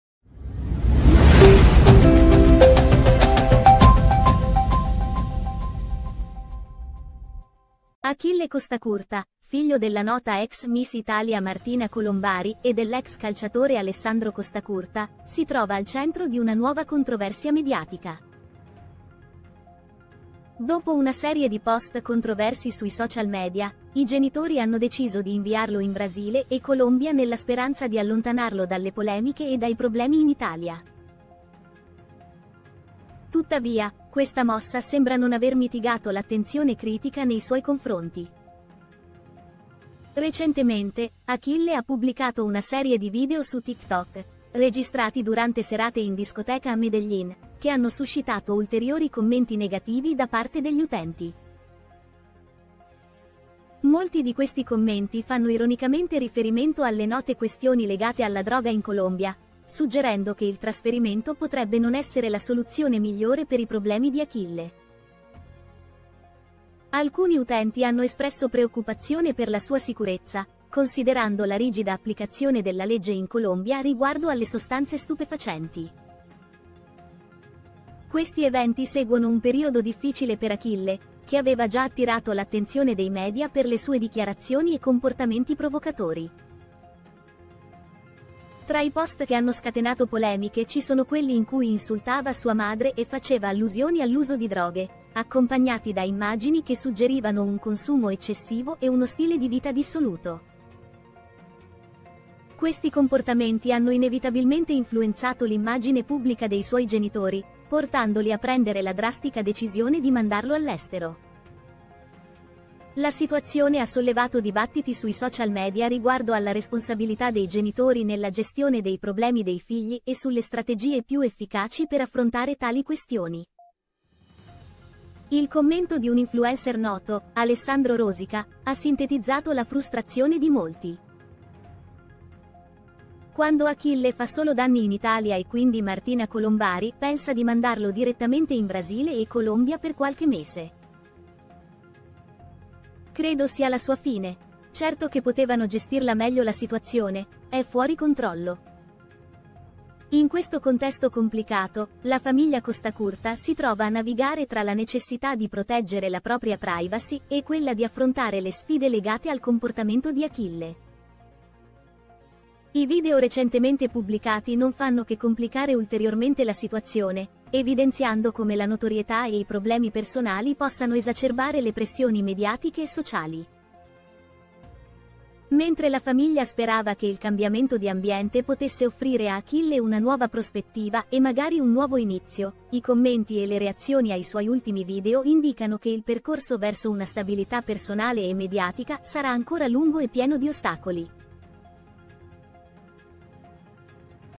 Tensioni in casa Costacurta “Genitori peggio di lui” Cos’è successo dopo la decisione di Martina
Achille Costacurta, figlio della nota ex Miss Italia Martina Colombari e dell’ex calciatore Alessandro Costacurta, si trova al centro di una nuova controversia mediatica. Dopo una serie di post controversi sui social media, i genitori hanno deciso di inviarlo in Brasile e Colombia nella speranza di allontanarlo dalle polemiche e dai problemi in Italia. Tuttavia, questa mossa sembra non aver mitigato l’attenzione critica nei suoi confronti.